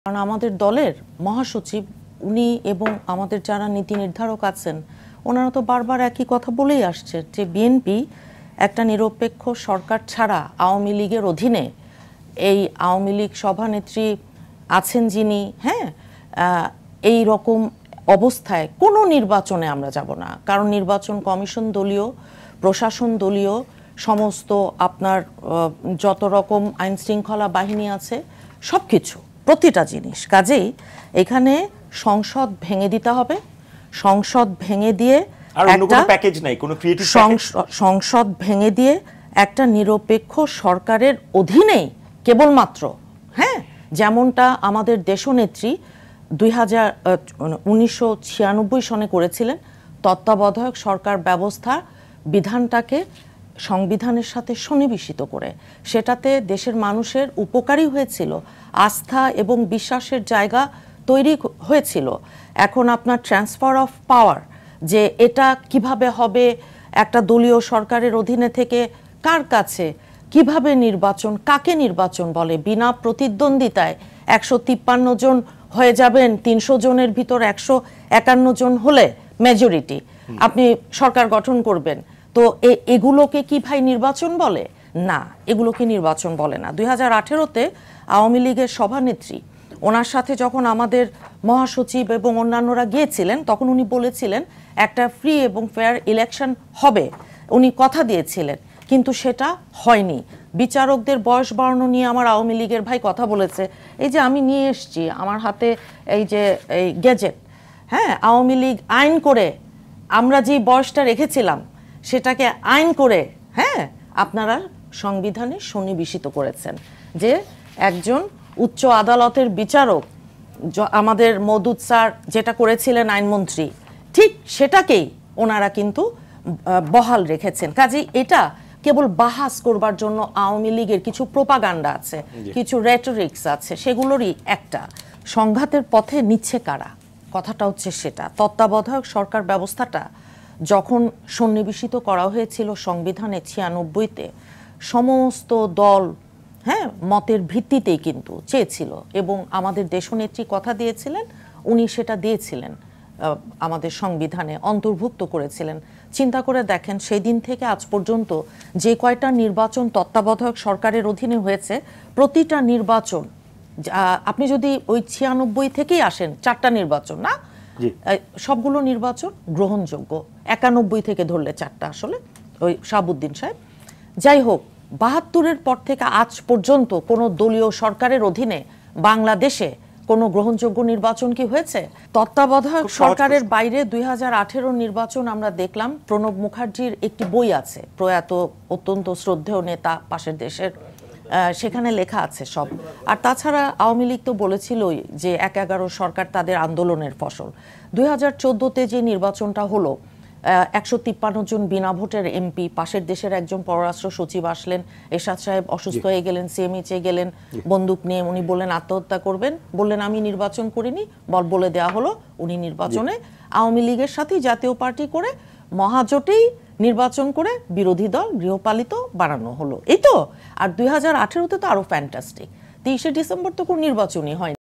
২০১৮ সালে আওয়ামী লীগ সভানেত্রী কথা দিয়েছিলেন ফ্রি এবং ফেয়ার নির্বাচন হবে - ফাহিমা নাসরিন মুন্নী |Rtv
২০১৮ সালে আওয়ামী লীগ সভানেত্রী কথা দিয়েছিলেন ফ্রি এবং ফেয়ার নির্বাচন হবে - ফাহিমা নাসরিন মুন্নী।

- Team RTV